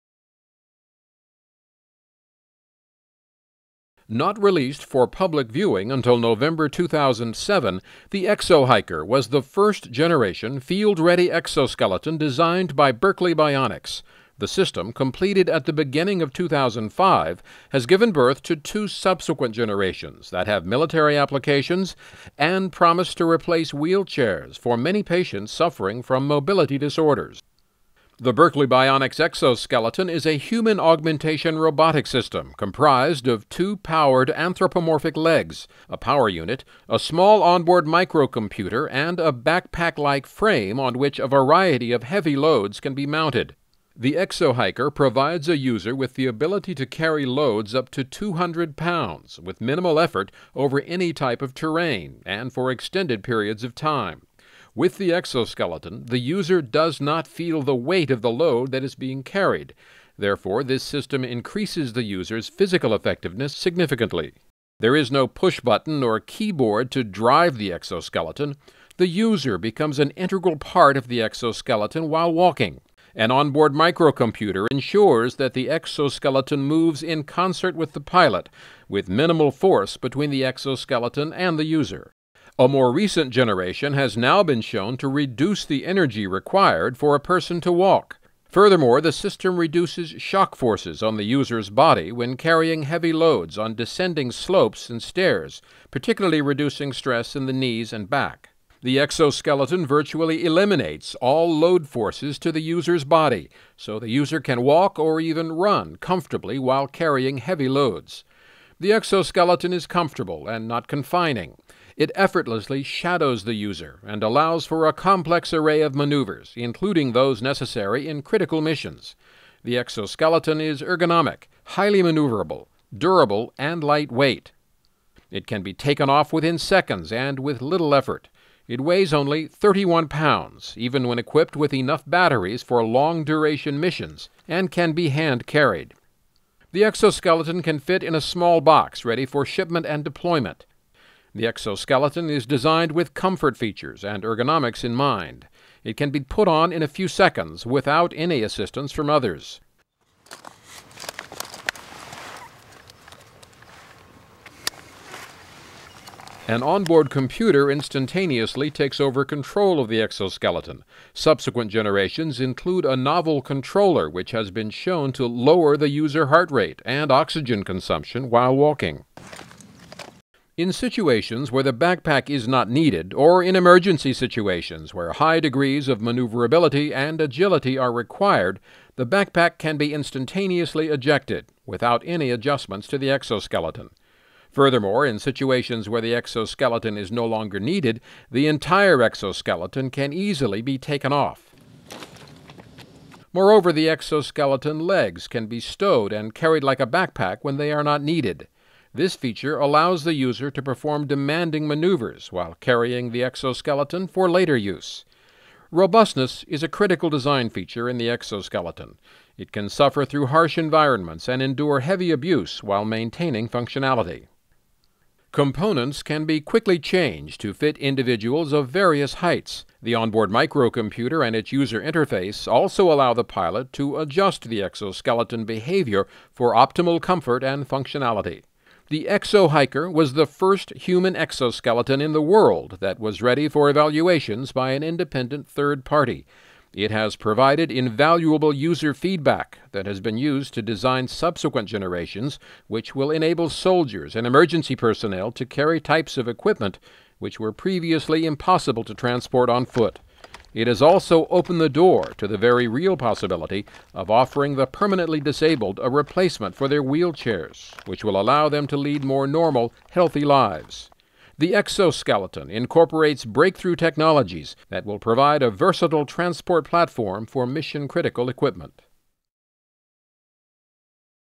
Berkeley Bionics Human Exoskeleton
Berkeley Bionics™, designs and manufactures lower extremity exoskeletons to augment human strength and endurance during locomotion.  Berkeley Bionics exoskeletons increase wearer's strength while decreasing their metabolic cost of walking. The company is also attacking the technological barriers to a practical, affordable exoskeleton for civilian and medical applications, particularly to assist patients with neurological or muscular mobility disorders.  These powered human exoskeletons would allow their wearers to walk upright without the strain and muscular effort required by today's unpowered orthotic devices.